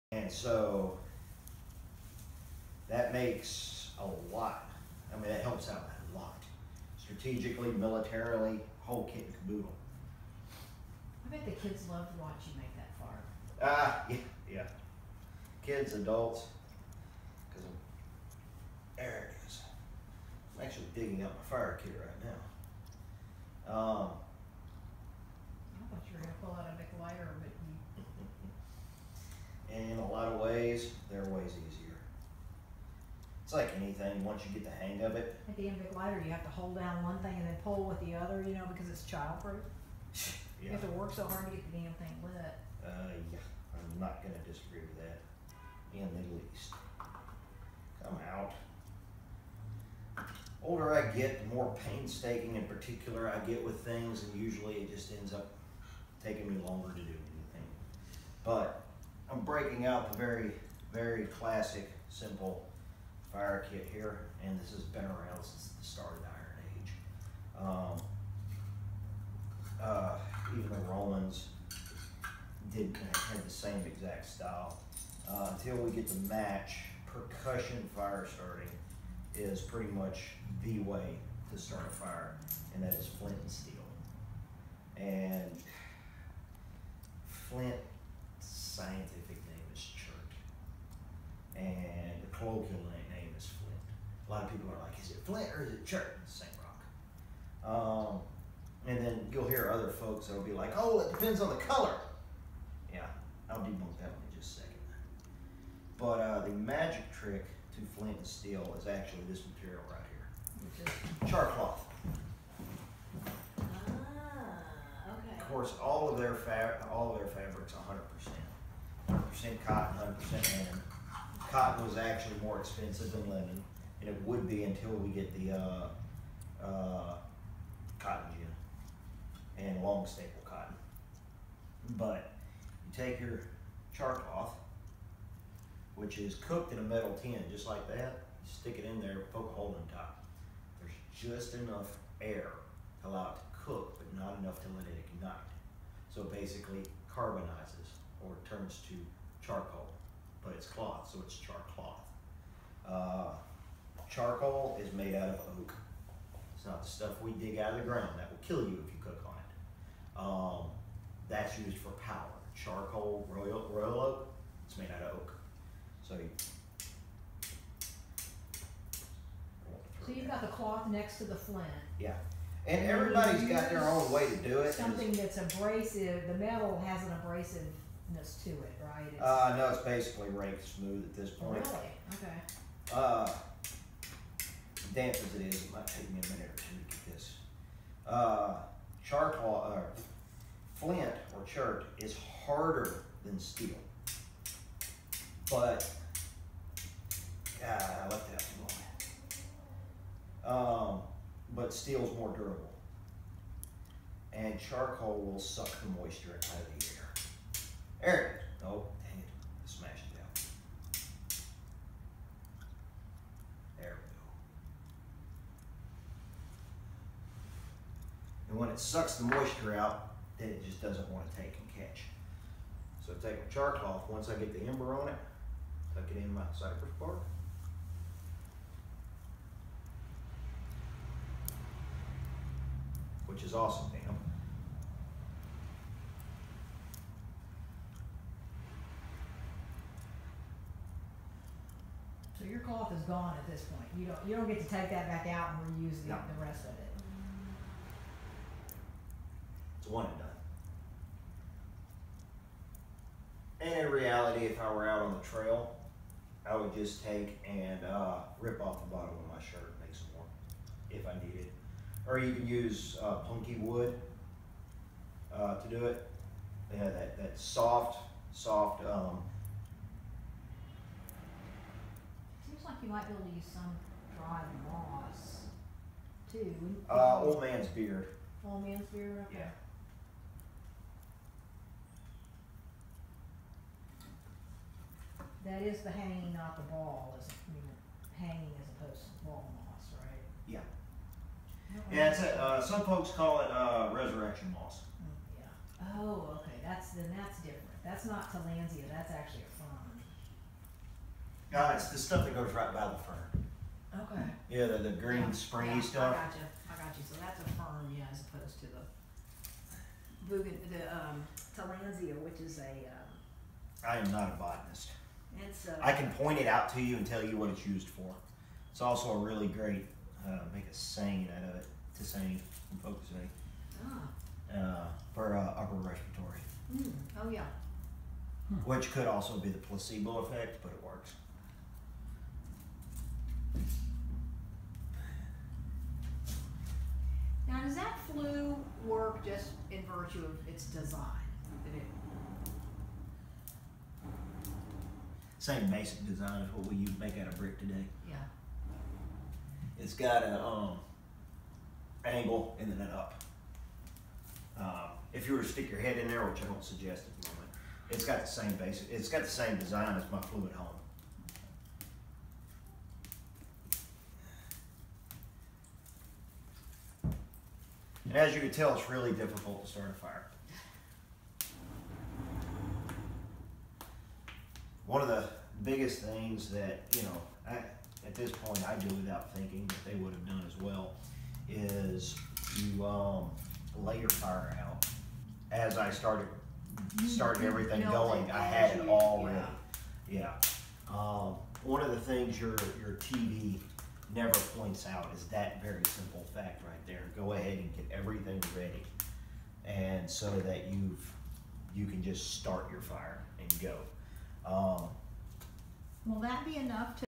How to light a fire
At the Fort St. Jean Baptiste State Historic Site in December 2016 we got a primer on how to start a fire without matches.